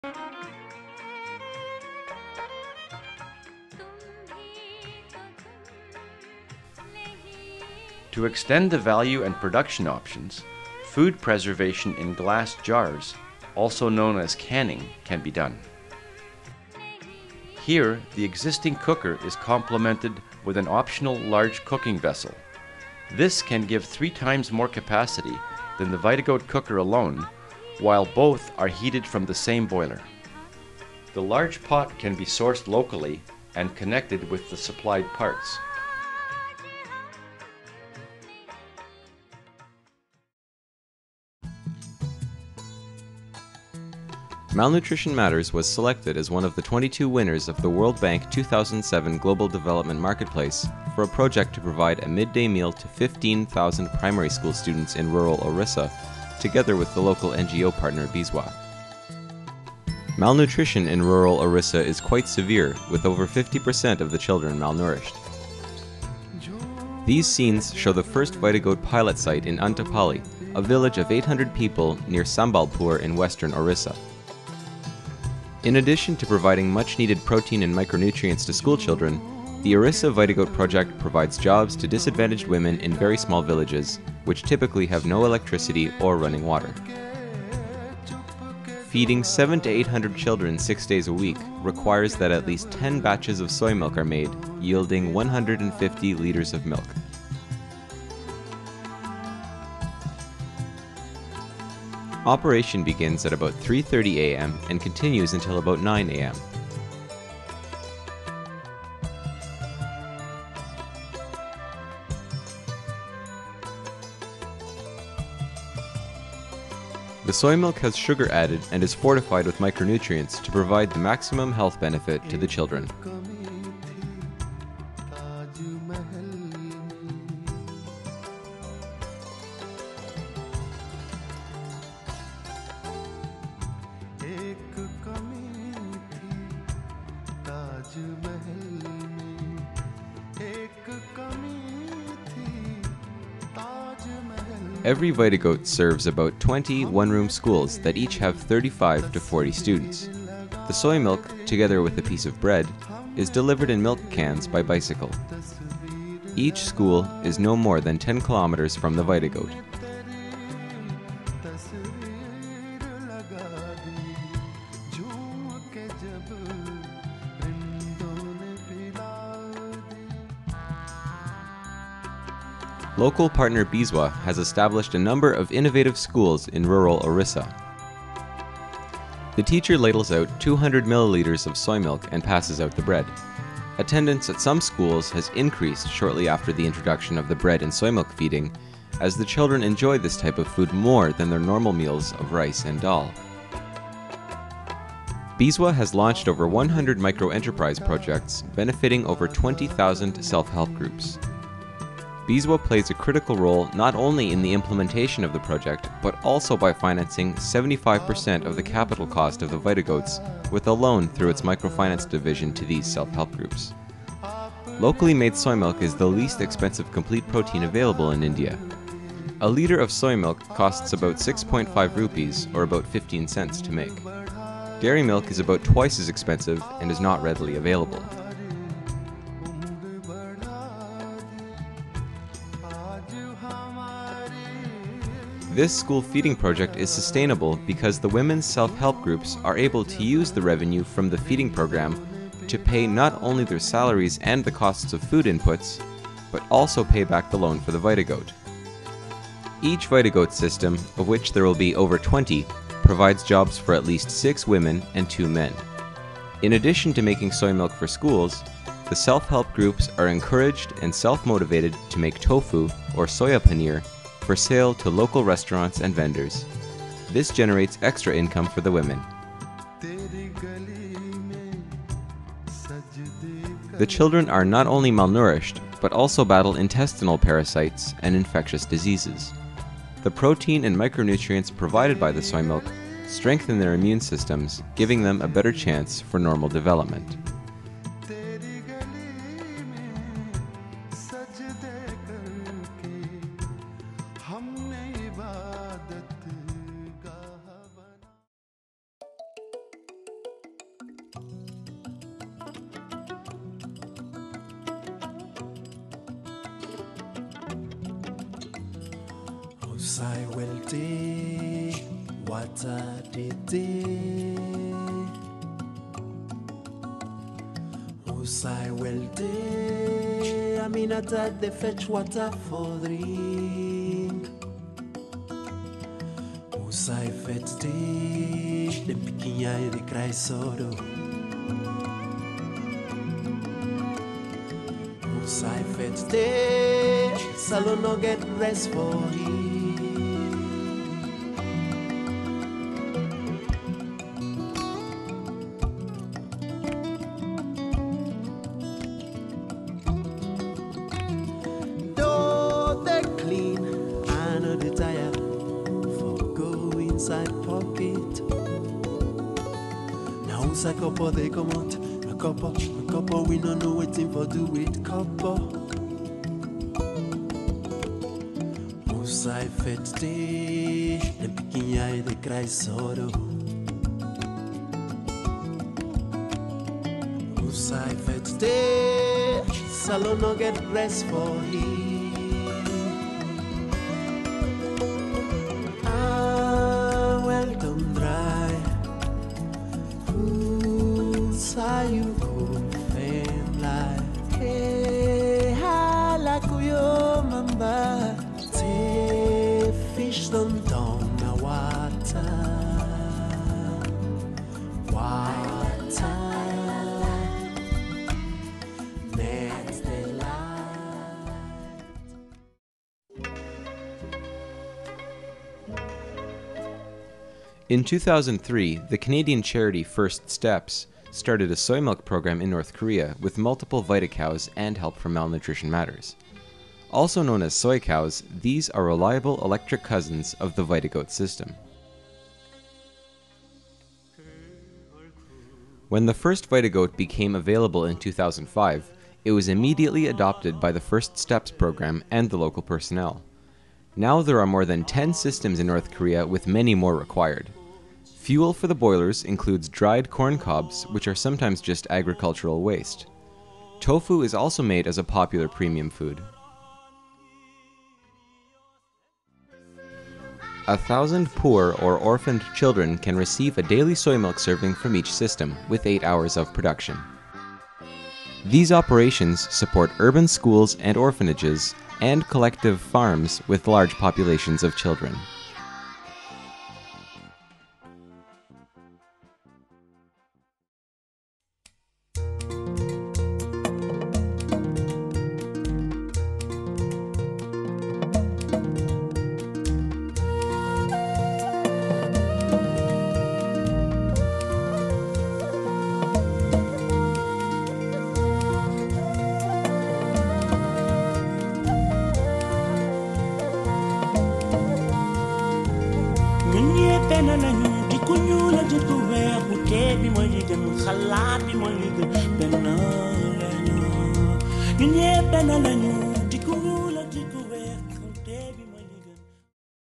VitaGoat Demo, Part 2
Introduction to the VitaGoat system, highlighting recent projects in Africa, India, and North Korea.